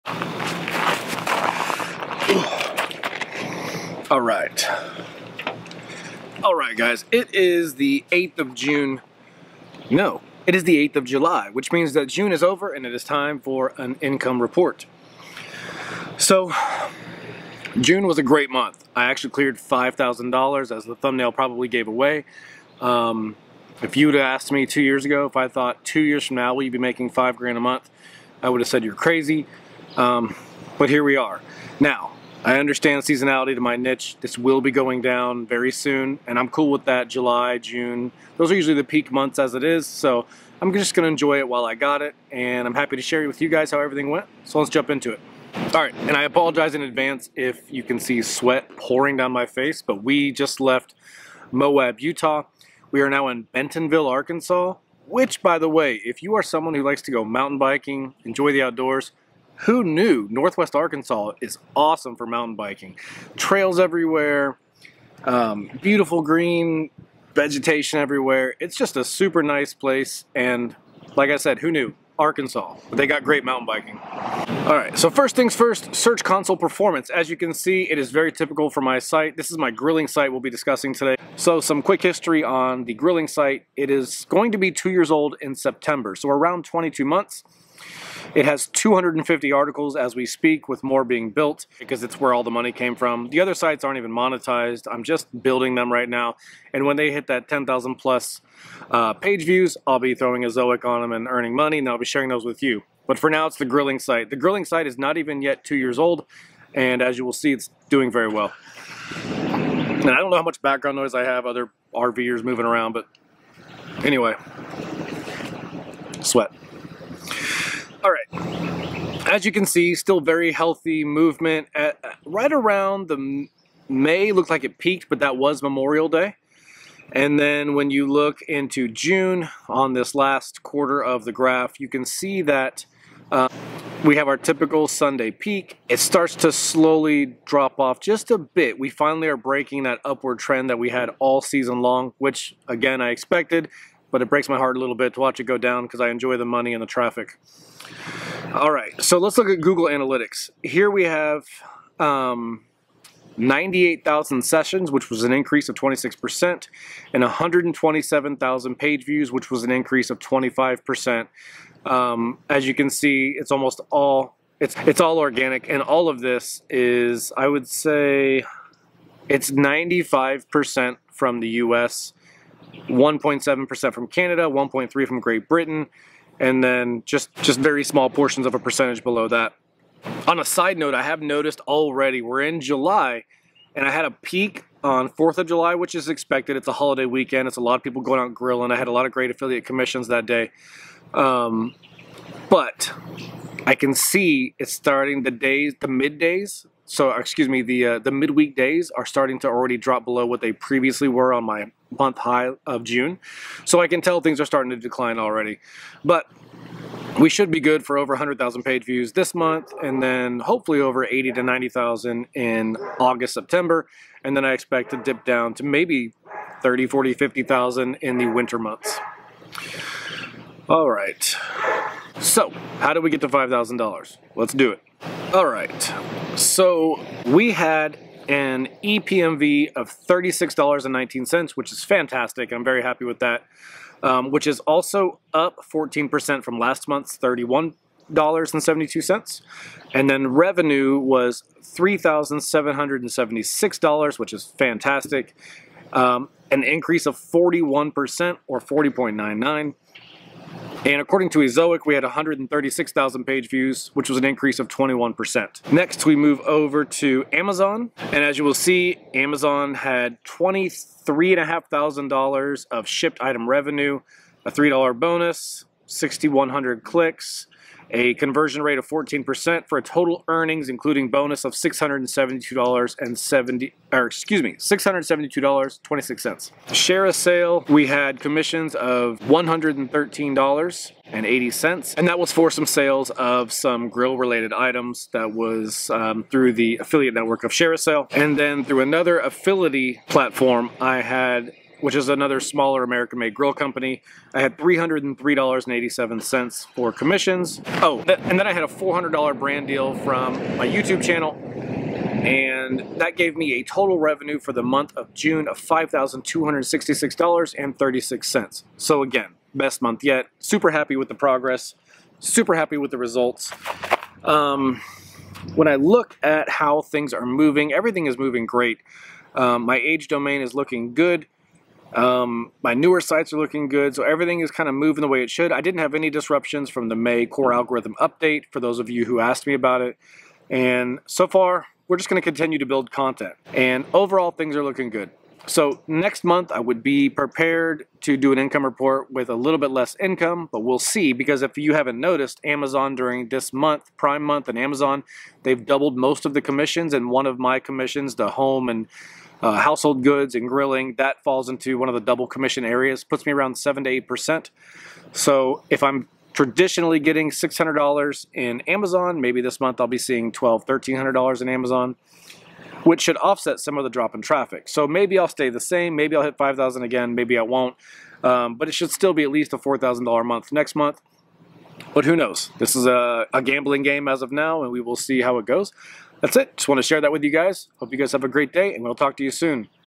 Niche Website Blogging Income Report June 2022
Here I share my niche website (blog) earnings for the month of May. Everything is up and the seasonality of this niche is really shining right now.

The Best Tool For Interlinking I have Found to increase Google’s Awareness of your site is Link Whisper: